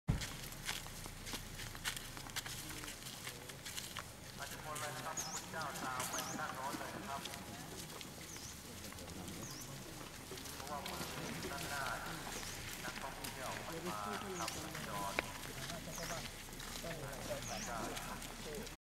ย้อนรอยจาริกธุดงค์ M2U02122
ย้อนรอยจาริกธุดงค์
ย้อนรอยจาริกธุดงค์ นำคณะจาริกธุดงค์โดย
พระอาจารย์ จรัน อนังคโน
ประธานสงฆ์ แห่งอุทยานธรรมดงยาง/วัดดงก้าวกัลยาราม
บ้านคลี กลิ้ง ต.คลี กลิ้ง อ.ศิลาลาด จ.ศรีสะเกษ